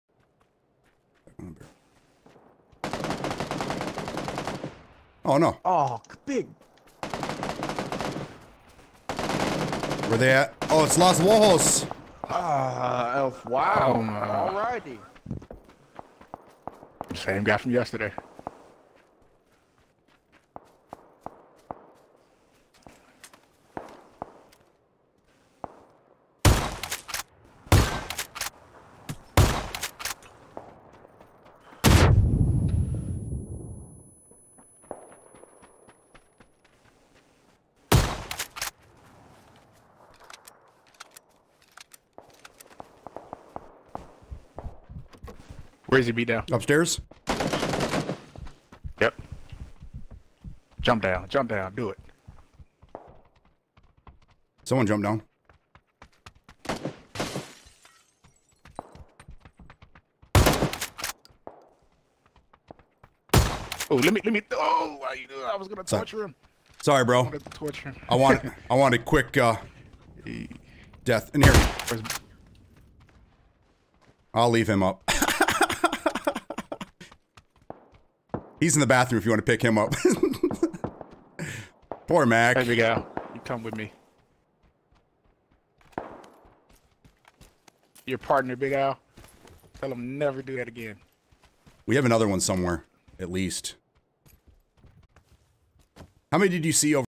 Trolling BigAlMac
BigAlMac ran random squads and ended up with our sworn enemy -- Watch live